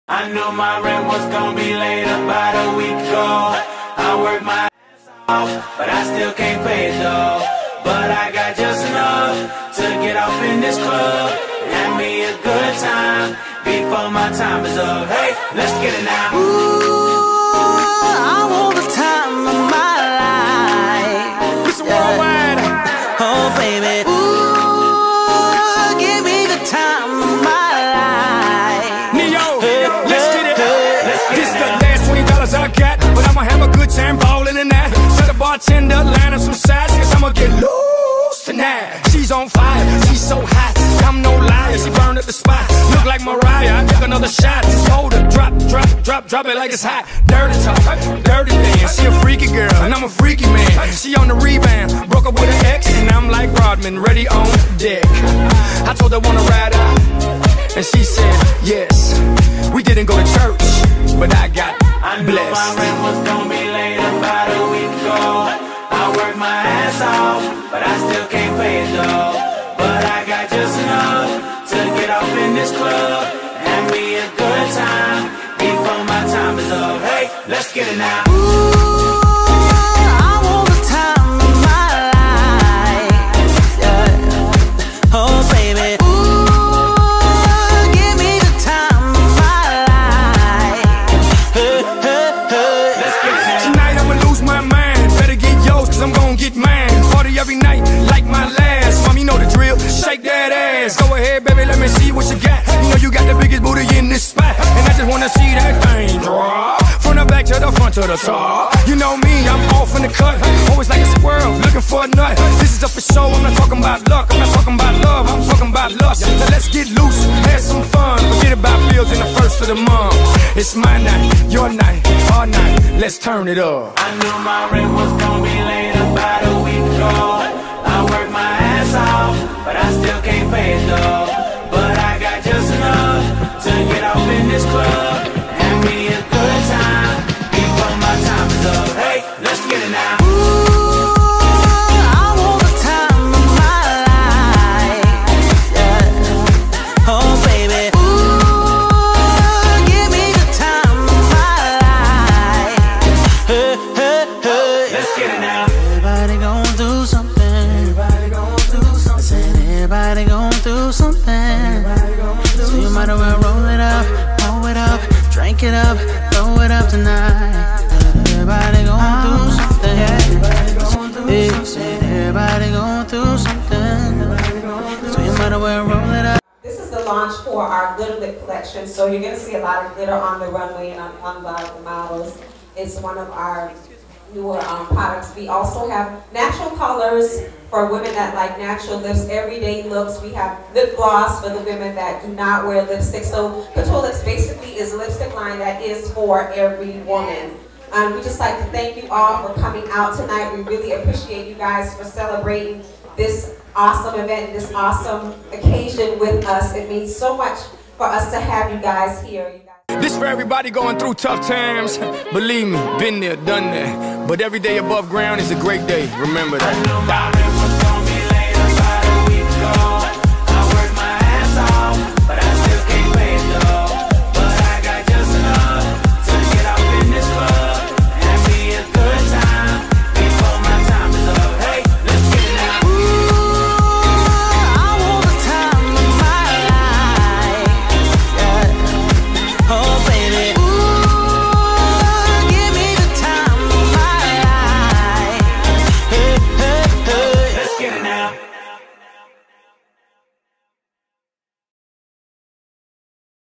The Barbie Twins Couture Lips Branch Launch Party & Event Mixer
EyeonFashion The Barbie Twins Couture Lips Brand Launch Party & Event Mixer. Presented by Delpha and Delerice of @ddcouture_couturelips.